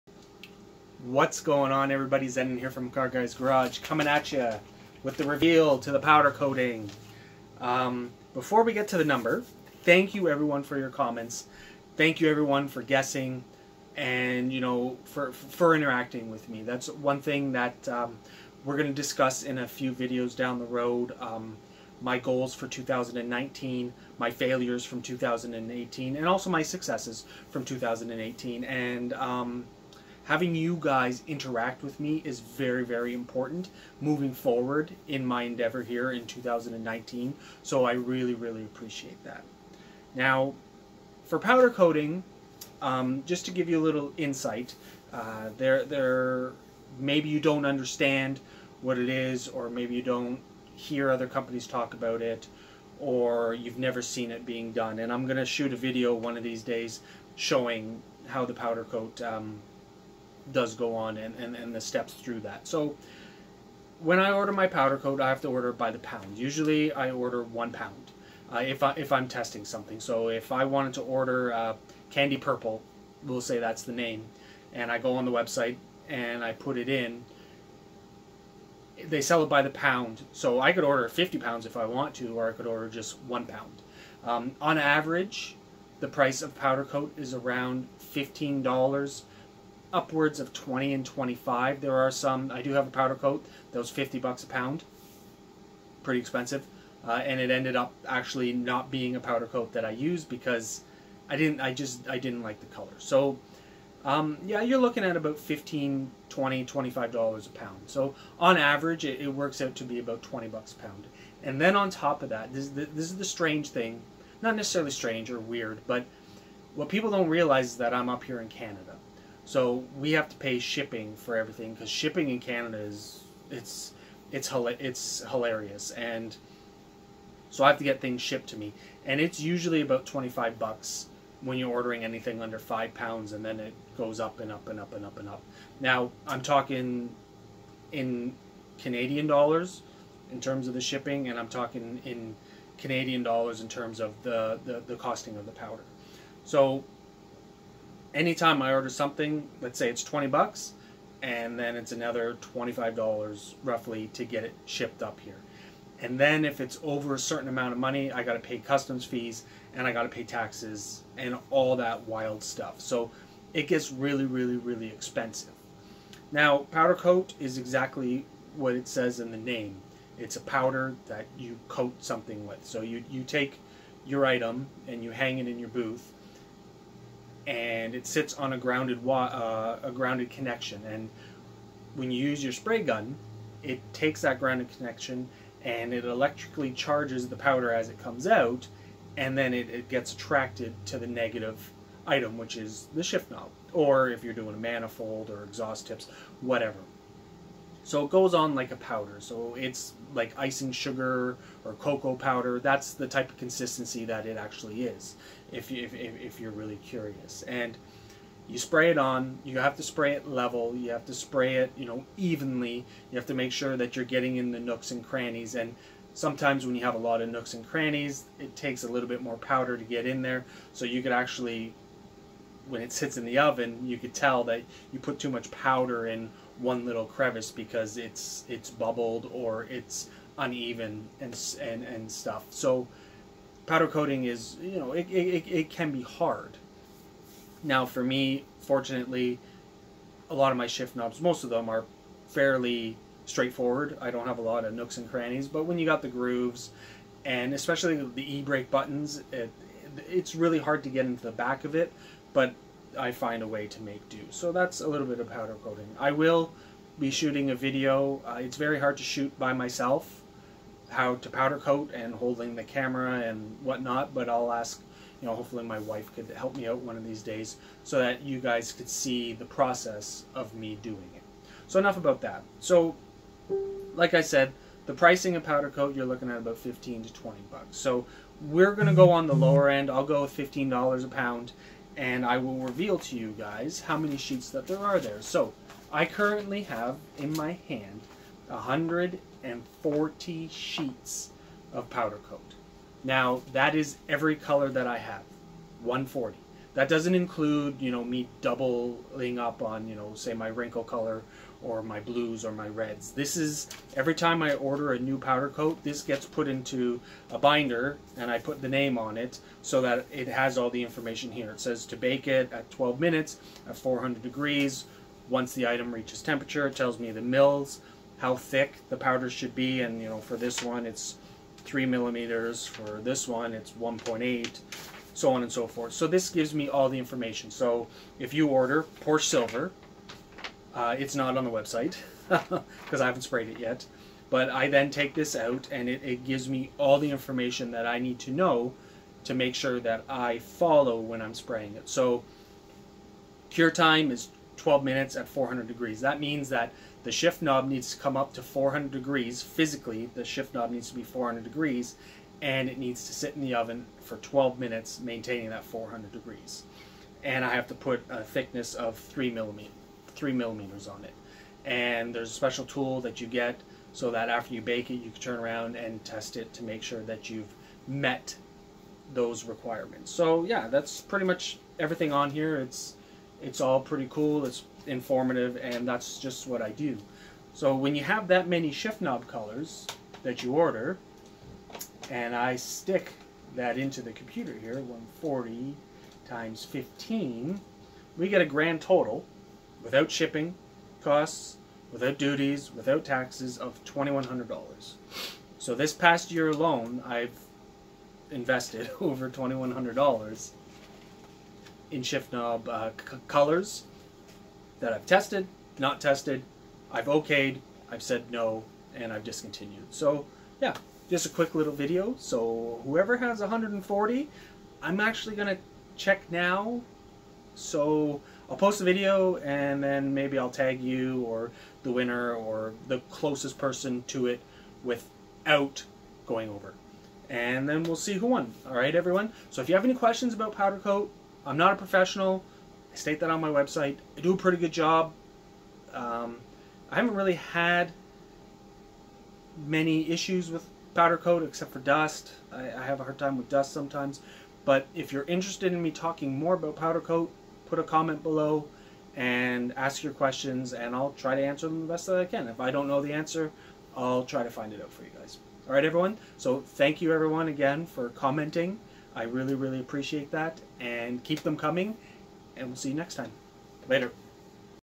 How many powder coat colors have I ordered?
Here we go the video that says how many colours I have ordered this year. There was one person who was only 2 numbers away.

Congratulations to them! I will contact them after video has aired for a bit.

Video also outlines some info and also rough cost for the powder. After the video I decided to add up all the bills from powder and it totaled $3100+/-

Really puts things into perspective now doesn't it??

Any questions or comments you know where to drop them. Thank you everyone for playing👌